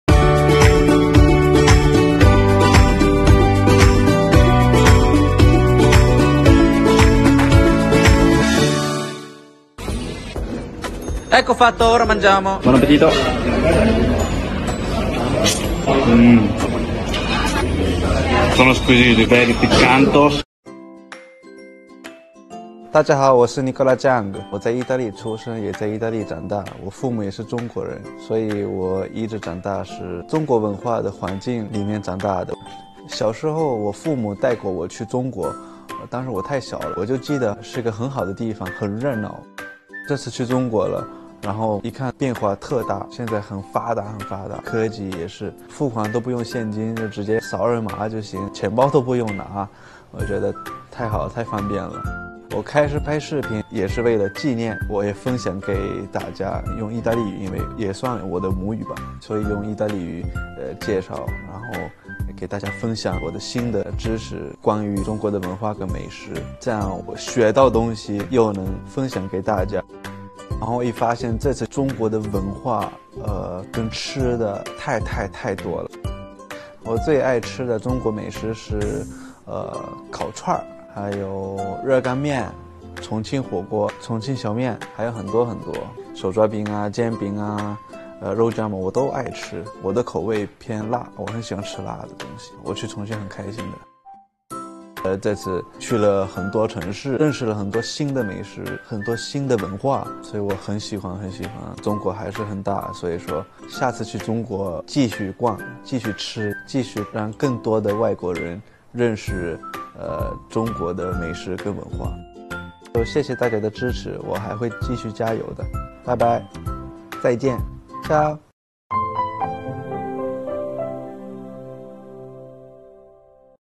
带着50万粉丝在中国逛吃的华裔博主
他在西双版纳吃炸竹虫、在重庆带着网友“云吃”重庆小面、还跟着温州当地渔民出海捕捞回各种小海鲜……本期“华裔青年说”，Nicola Jiang讲述他的“美食之旅。”